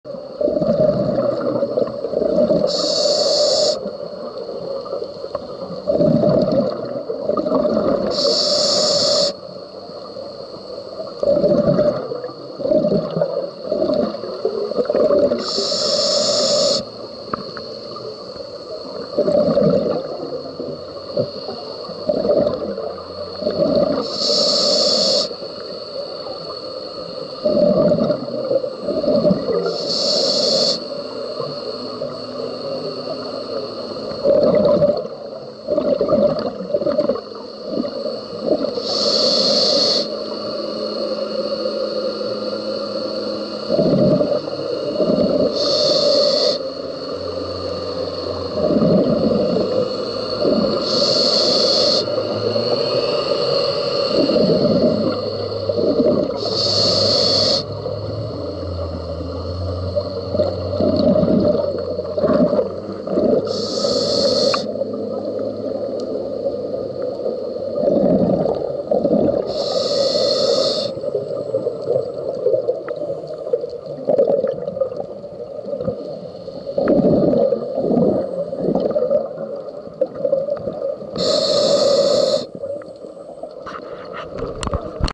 Salem Express wreck 2015 (Sony HDR-AS200V)
uszkodzenia na dziobie i burcie po wpadnięciu na rafę